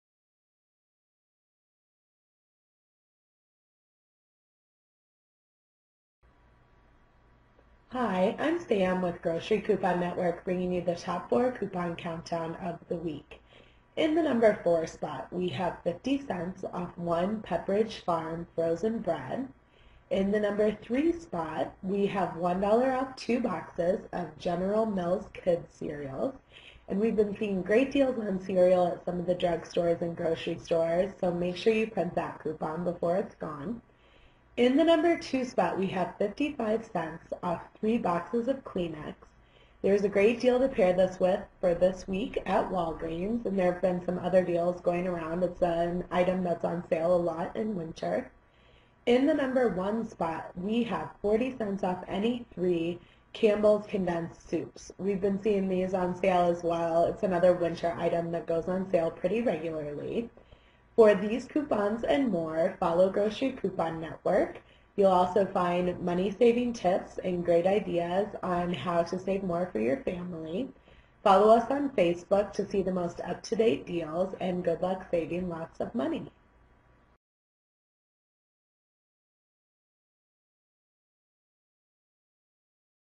Top 4 Coupon Countdown - January 5 - 11, 2013
1. $0.40 off any (3) Campbell's Condensed Soups

2. $0.55 off THREE boxes of KLEENEX Facial Tissue

3. $1.00 off TWO BOXES General Mills kid cereals

4. $0.50 off any (1) Pepperidge Farm frozen bread